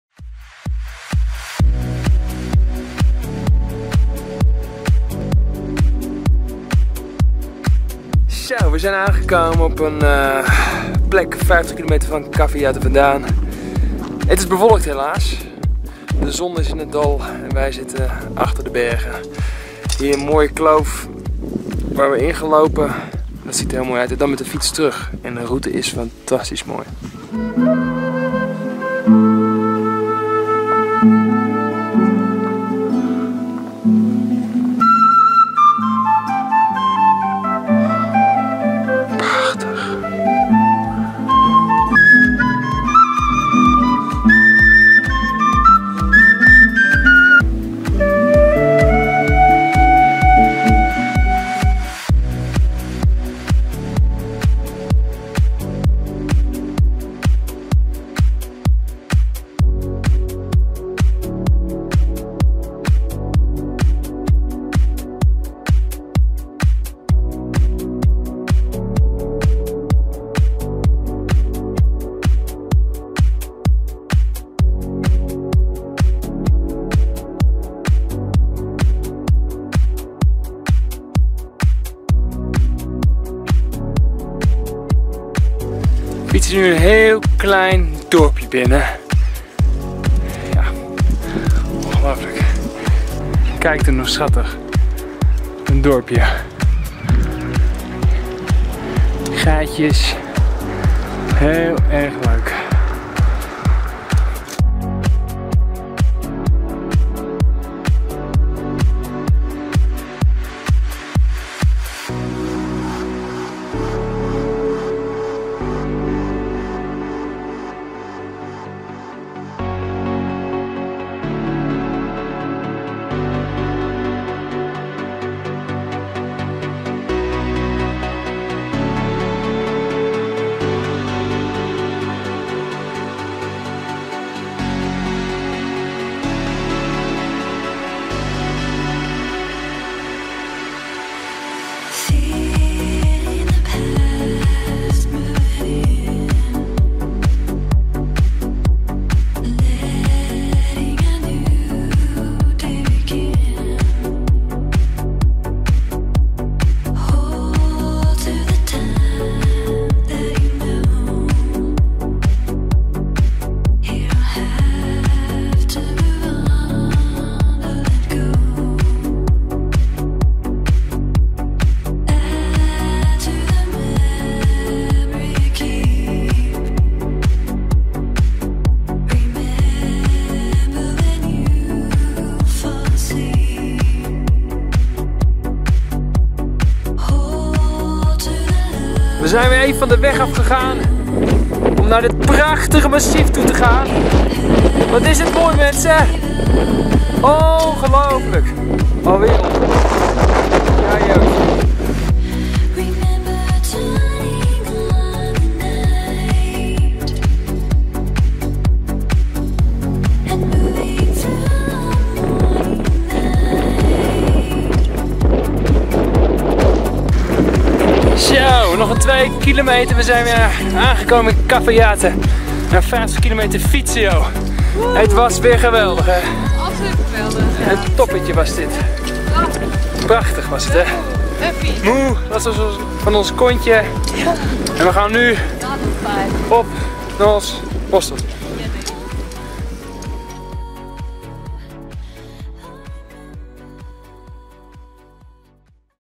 Quebrada De Las Conchas - Cafayate Argentina
With our bikes in the trunk the bus dropped us of somewhere 50 km from Cafayate. We cycled the way back through beautiful Quebrada De Las Conchas. A definitely must do in North Argentina.